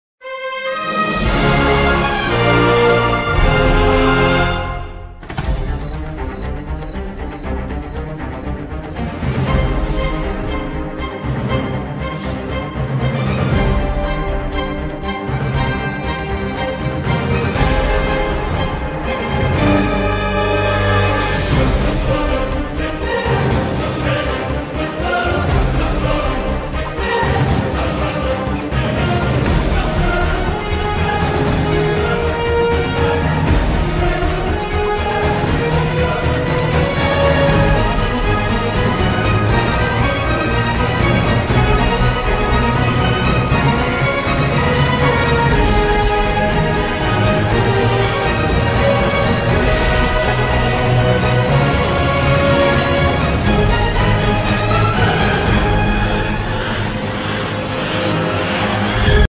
New 2022 Fisker Ocean | Ultimate Design SUV
Fisker Ocean Coming This Summer | $37,499 EV with 300 miles long driving range

➤ You can also visit the online store of the best Tuning Parts you can get at CHILINGTUNING